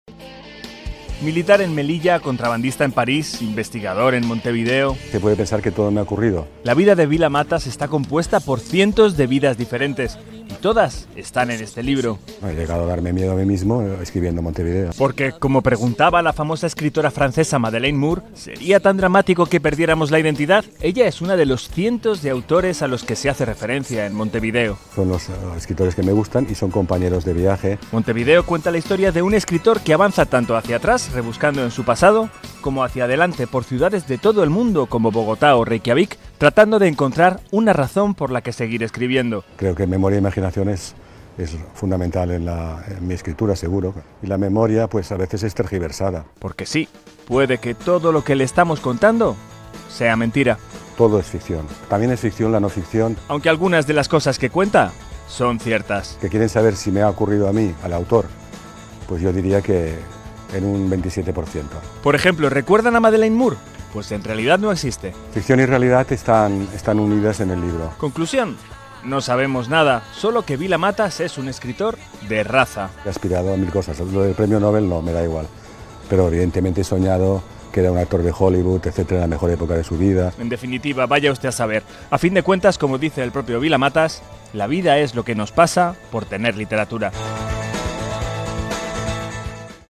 Enrique Vila-Matas: "He llegado a darme miedo escribiendo 'Montevideo'"
El escritor barcelonés vuelve a las librerías con 'Montevideo' (Seix Barral, 2022) una obra que, dice, "le ha impresionado a sí mismo".